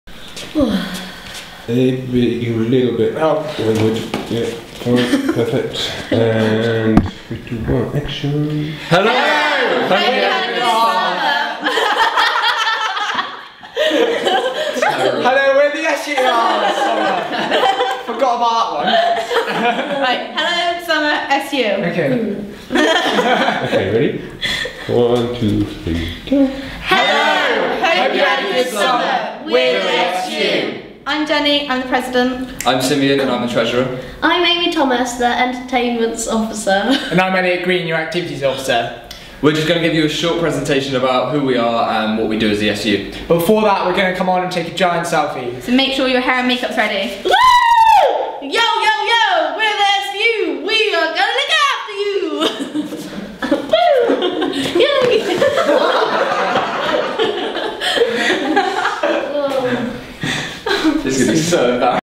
SU welcome video, Bloopers X2
Filming the SU welcome video took a while to get right. Heres a short video to show you.